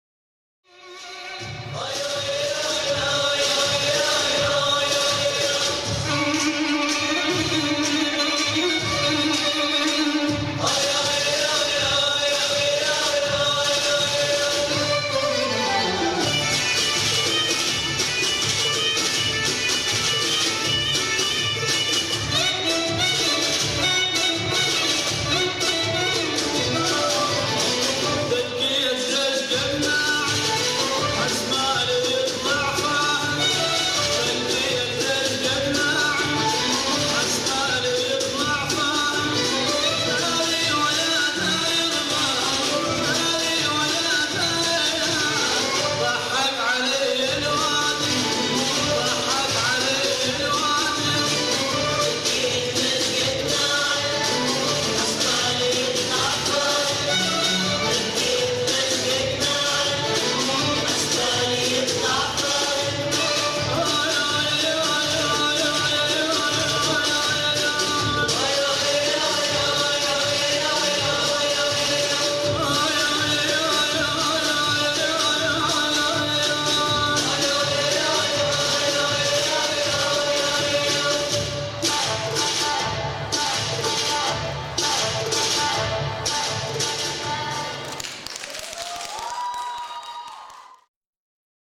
Daniella Oleinik ⊰⊱ V. Feeriya '12.
Даниэлла Олейник на 6 фестивале "Восточная Феерия 2012".
21.04.12. Днепропетровск. Дворец Культуры Машиностроителей
(Юниоры 1 Н Фольклор) .